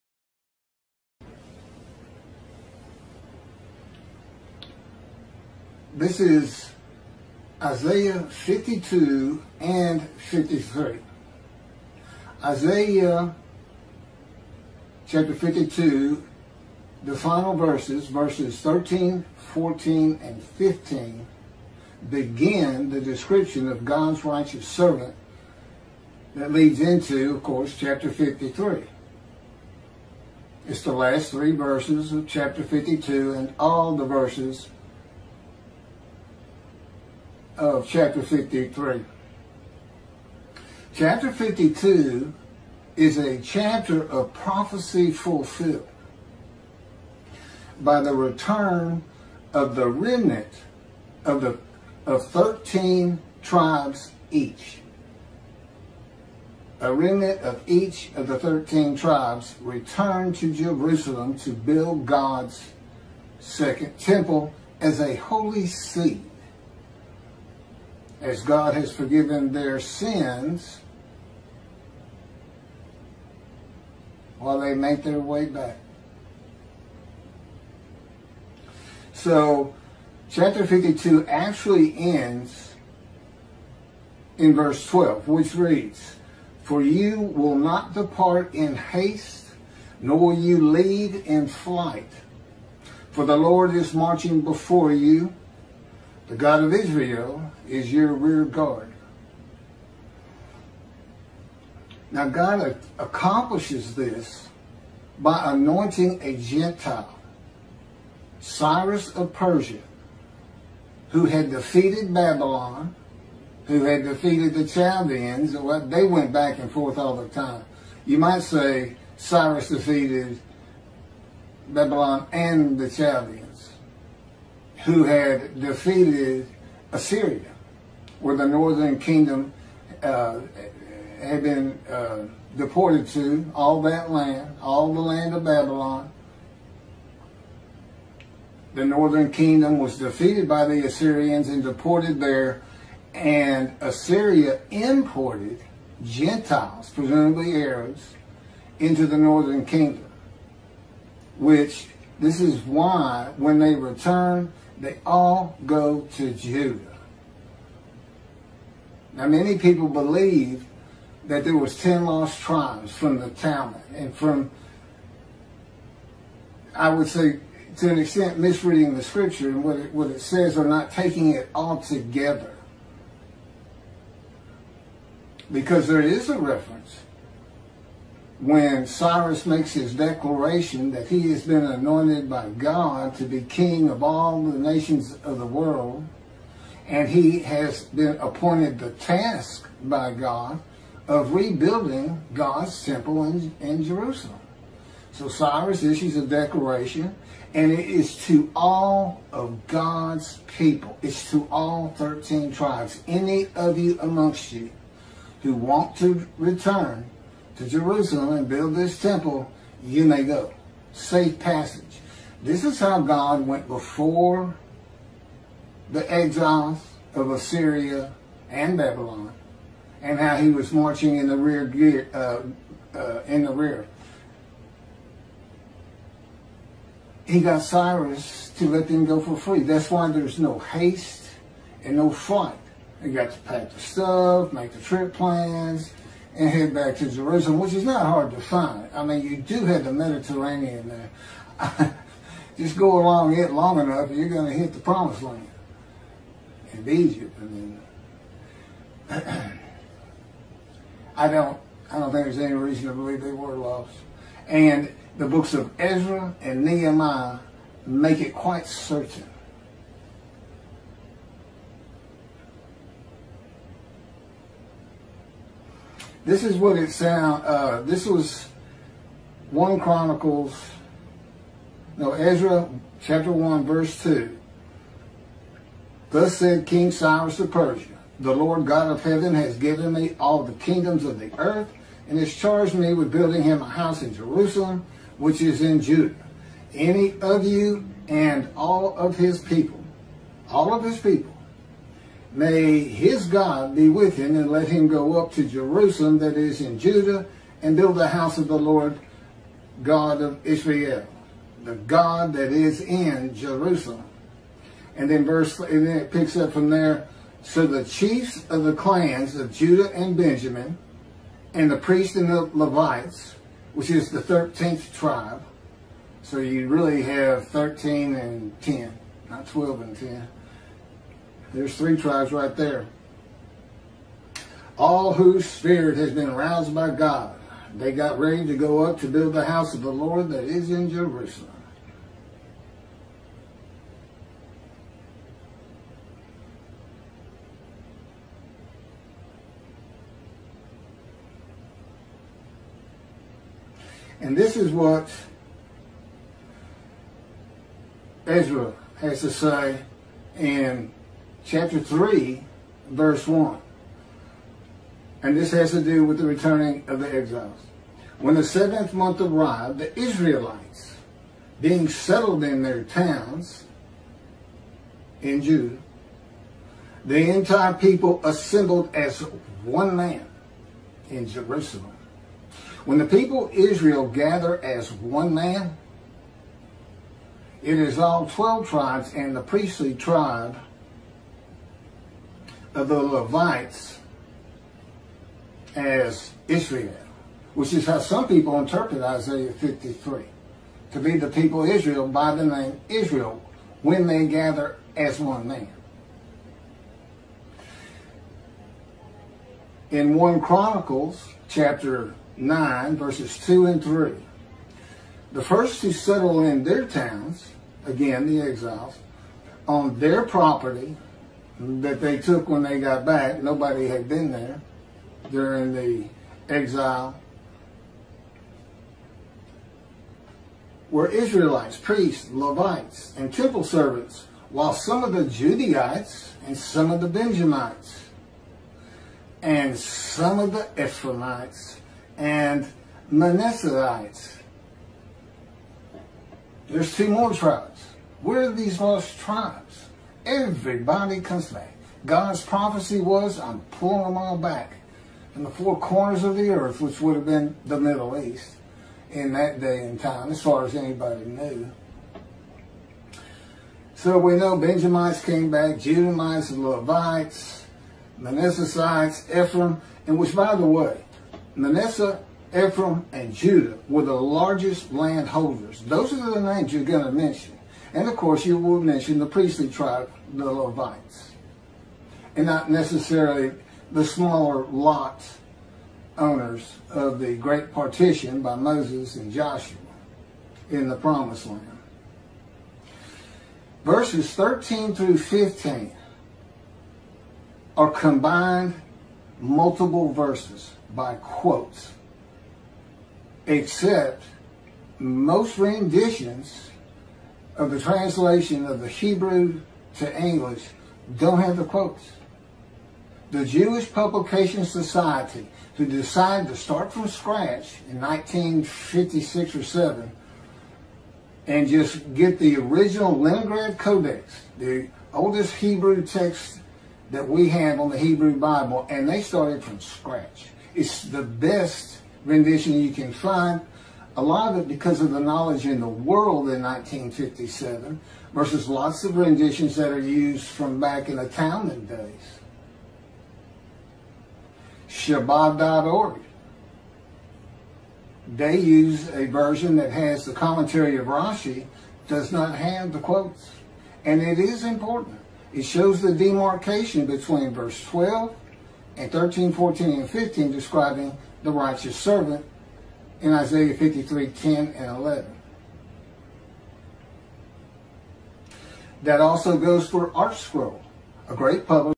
Isaiah 52 and 53 and the Multiple Verse Quotes. Why?????
In Isaiah 52/13-15 the Lord begins to describe His righteous servant of Chapter 53. Isaiah 52/13-15 should have been verses 1-3 of Chapter 53

Isaiah 52, a chapter of prophecy fulfilled by the return of the remnant of the thirteen tribes of Israel God often calls His servant, concludes with verse 12.

12For you will not depart in haste, Nor will you leave in flight; For the Lord is marching before you, The God of Israel is your rear guard.

The remnant of each of the thirteen tribes did not leave Assyria/Babylon/Persia in haste and flight. God marched before them and was their rear guard by the anointment of the gentile Cyrus of Persia as King of all the nations of the world, with the task of building God's Second Temple in Jerusalem. To do this Cyrus issued a Declaration:

Ezra 1/2 “Thus said King Cyrus of Persia: The Lord God of Heaven has given me all the kingdoms of the earth and has charged me with building Him a house in Jerusalem, which is in Judah. 3Anyone of you of all His people—may his God be with him, and let him go up to Jerusalem that is in Judah and build the House of the Lord God of Israel, the God that is in Jerusalem; …….

5So the chiefs of the clans of Judah and Benjamin, and the priests and Levites, all whose spirit had been roused by God, got ready to go up to build the House of the Lord that is in Jerusalem.

It is said in writings by Sages and Rabbi’s that ten of the twelve tribes of Israel became lost and did not return to Judah to build the second Temple. There never were lost tribes according to the Hebrew Bible. If there were lost tribes the many accounts of their return in Ezra, Nehemiah, and 1 Chronicles were not true, and Isaiah wrote a prophecy of God that was not fulfilled.

The tribes of Reuben, Gad and the ½ tribe of Manasseh that settled outside of the promised land east of the river Jordan, and the tribes of the northern Kingdom of Samaria (also called the kingdom of Ephraim and the kingdom of Israel) were defeated by the Assyrians and deported to lands in Assyria north-west of Babylon (Iraq) and to the towns of Media (Iran). The Assyrians imported gentiles to the lands of the northern kingdom.

The Kingdom of Judah was defeated by the Babylonians and in stages deported to Babylon (Iraq). Jerusalem is within the lands of Benjamin which lands are considered part of the Kingdom of Judah since that is where the Kings of the lands of Judah ruled from.

“When the seventh month arrived—the Israelites being settled in their towns—the entire people assembled as one man in Jerusalem.” Ezra 3/1
When the people Israel gather as one man it is all twelve tribes and the Levites (the priestly tribe without an allotment of the promised lands).

“The  first  to  settle  in  their  towns,  on  their  property,  were  Israelites,  priests,   Levites,   and  temple servants, while some of the Judahites and some of the Benjaminites and some of the Ephraimites and Manassehites settled in Jerusalem.” 1 Chronicles Chapter 9/2-3

Ephraim and Manasseh were not lost tribes as many believe from writings outside of the Hebrew Bible.

Verses 13, 14, and 15 are combined in quotes.

The translation of the Hebrew Bible by Artscroll and Chabad of Isaiah 52, with Rashi’s comments, and the translation used by Jews for Judaism in their commentary on Isaiah 52 and 53, do not include the quotations that combine verses 13-15.  I use the 1985 JPS, begun from scratch in 1956 translating the oldest Hebrew Bible, the Leningrad Codex.

They are the only quotations of Isaiah 52 and a demarcation of the verses of the fulfillment of prophecy by the return of the remnant of the thirteen tribes from exile.

They are the beginning of the description of God' righteous servant of Isaiah 53 and have nothing to do with the exiles, God's servant, the Jewish people.

God's righteous servant is a gentile, in the beginning.

Isaiah 63 says God comes from Edom that is interpreted in Judaism to be Christianity and means He is coming from a Christian country, and of the people (Jewish people) none are with Him. He comes with a gentile.

Jesus was a Jewish man who came from Nazareth. The Jewish people did not come from Edom. They began in the promised land (today called Israel), returned from Egypt in the Exodus and were not allowed to pass through Edom, and returned from Europe after the Holocaust.

or:

Keithmccartymccarty.wordpress.com